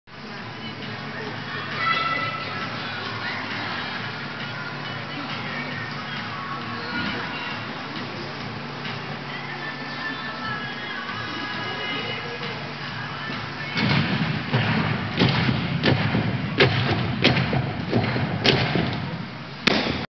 Oakville Gymnastics Club Tumbler Jack McGarr 2nd ON Cup 1st finals
Here is the Oakville Gymnastics Club's tumbler, Jack McGarr performing his 1st finals tumbling pass at the 2nd Ontario Cup for tumbling 2012.